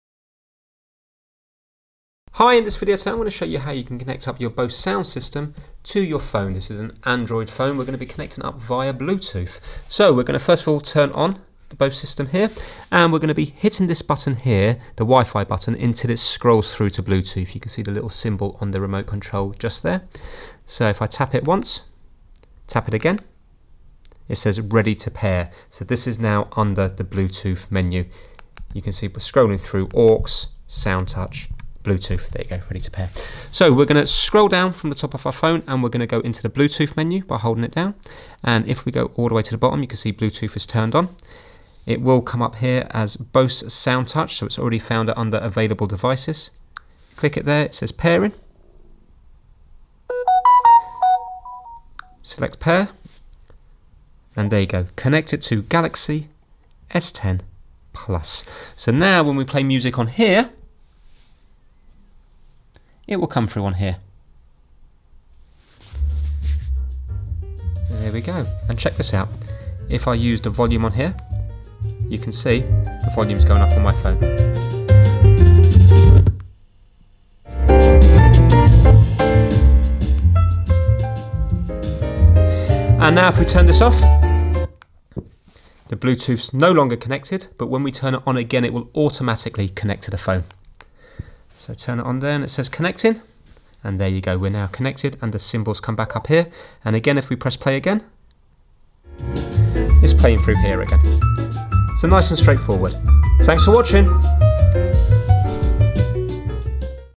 BOSE Wave Soundtouch 4 - How to Connect Android Phone via Bluetooth
In this short video we show you how to connect your Android device to the Bose Wave Soundtouch 4 sound system.